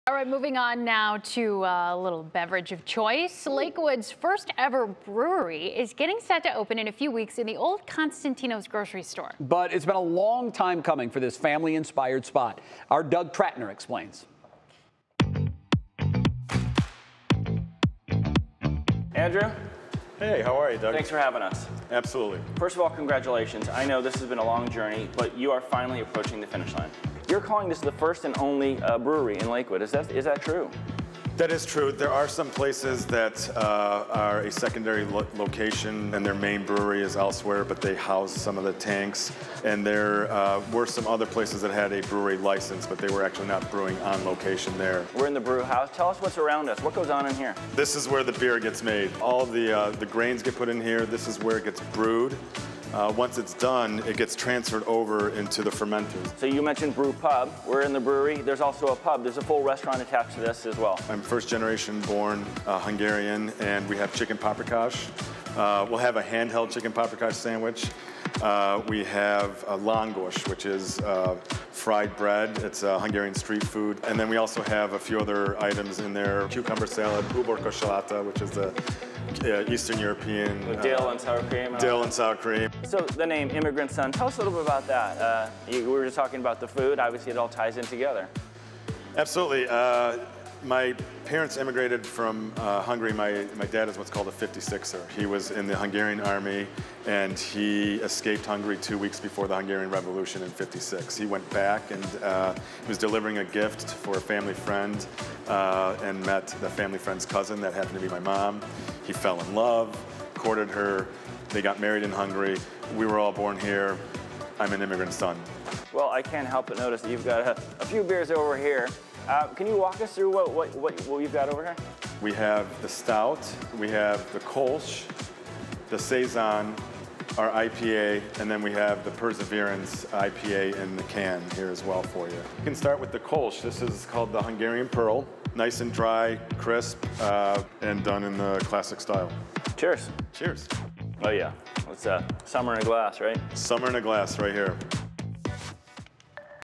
Immigrant Son Brewery opening soon as Lakewood's first-ever brewery
Lakewood residents and beer lovers alike have been impatiently waiting for Immigrant Son Brewery to open in the city's West End neighborhood. Now, the ambitious project — more than two years in the building plus more in the planning — is finally approaching the finish line, and ttaps should start flowing some time at the beginning of October.

When the former Constantino's Market closed, Andrew Revy saw a silver lining. The 9,000-square-foot building would make an ideal brewery, the owner thought, thanks to its solid foundation, spacious interior and generous parking lot.

Efforts were made to retain original features such as skylights, exposed timber framework, windows and even the walk-in dairy cooler. The airy main dining room boasts cathedral ceilings, a 15-seat bar, booth seating and casual high-top lounging. A patio will also be added down the road.